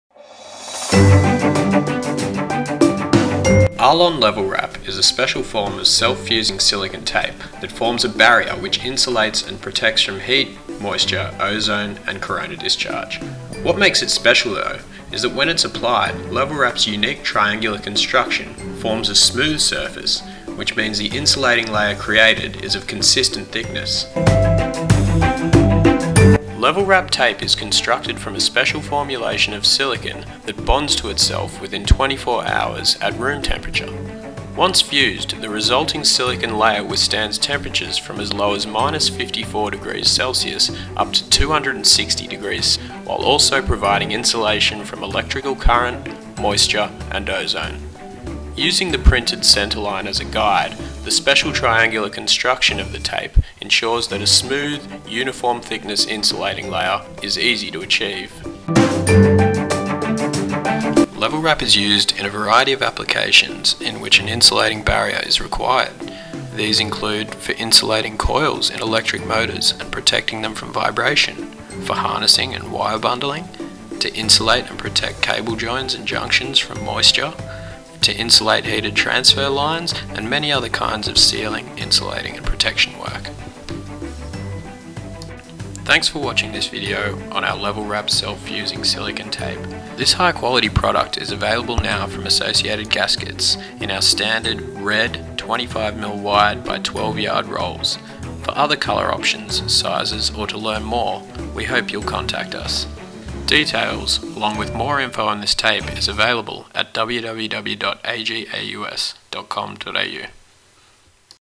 LevelWrap Self Fusing Silicone Tape
Level Wrap is a special form of self-fusing silicone tape that is manufactured with a unique triangular profile construction with tapered edges. This enables the tape to form an insulating barrier of uniform thickness that will protect from heat, moisture, ozone and electrical corona discharge.

Watch the video find out more...